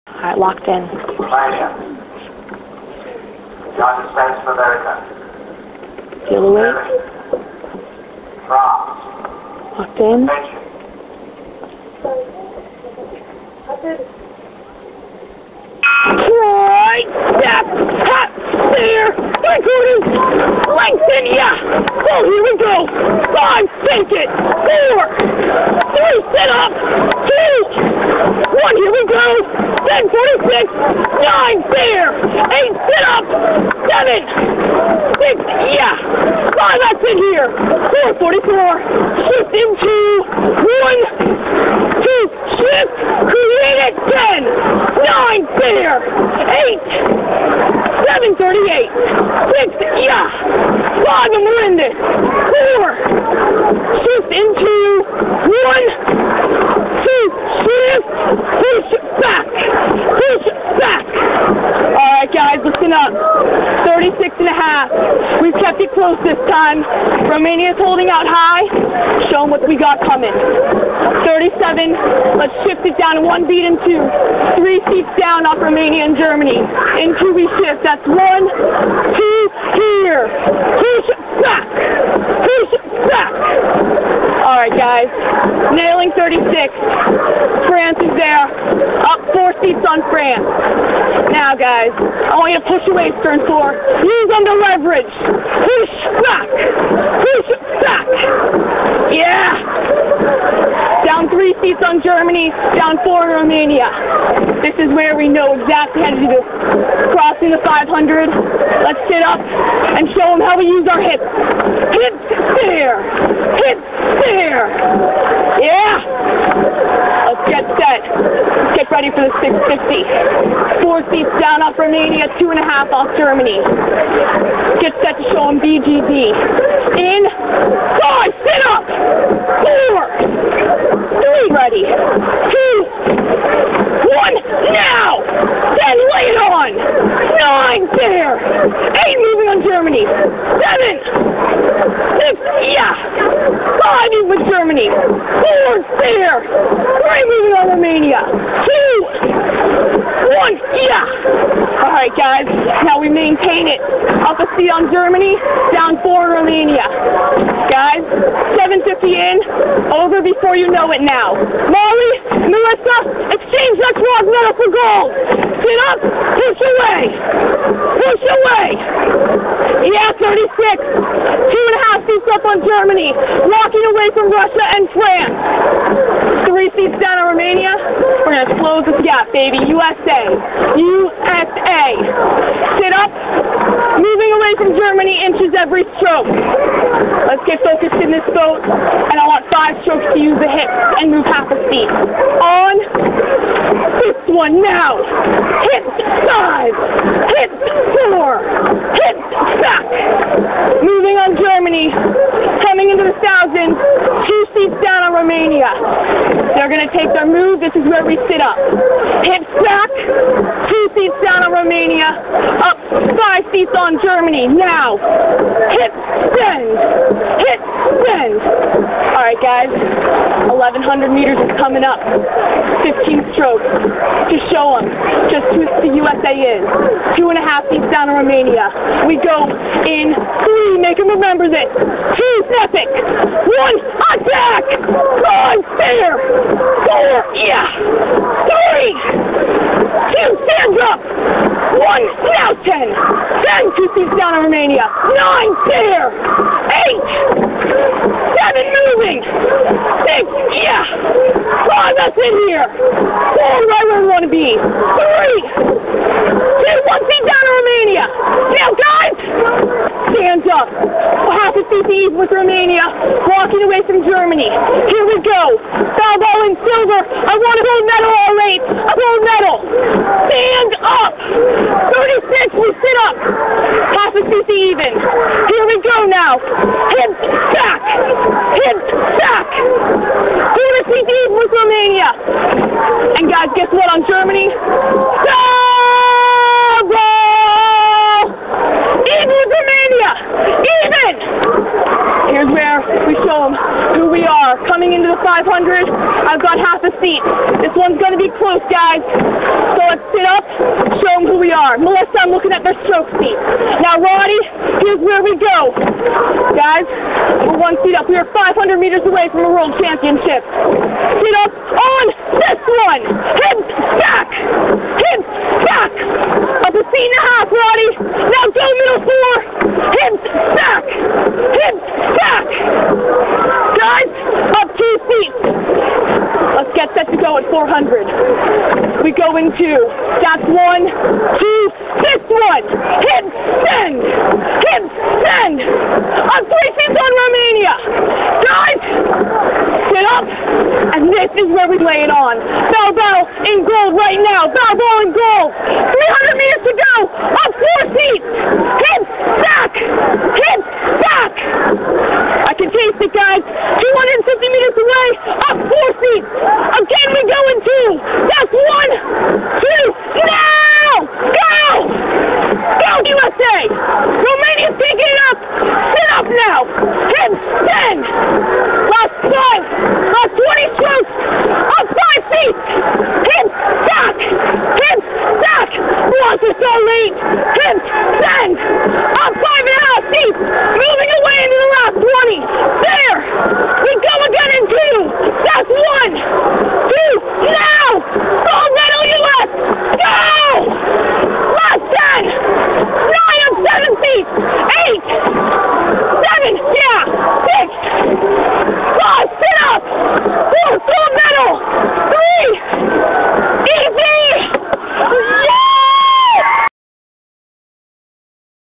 Chelsea Lucas 2008 World Championships USRowing Junior National Team (Coxswain recording)
Credit to USRowing. Reposting here because the download link no long works on the USJNT website.